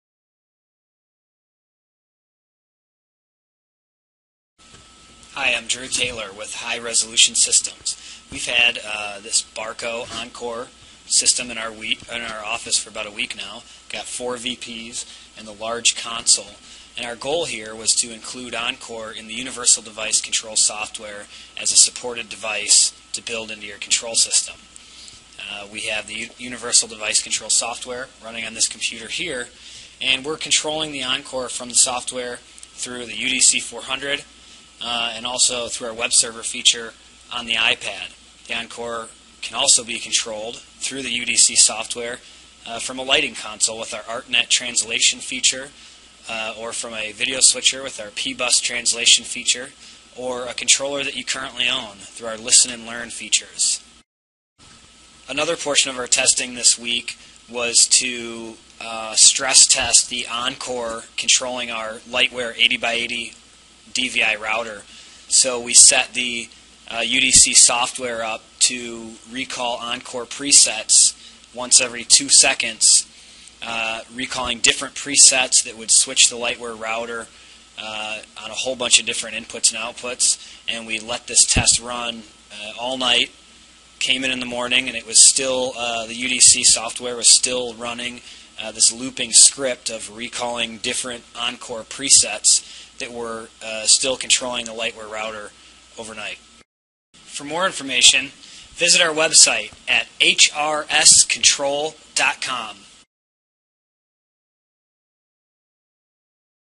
UDC Barco Encore Test
High Resolution Systems has a Barco Encore system in the office for the week in order for testing and implementation in the Universal Device Control software or UDC software for short. Control of the Barco Encore Presentation System is shown from the UDC 400, iPad and other controllers and switchers through the UDC software.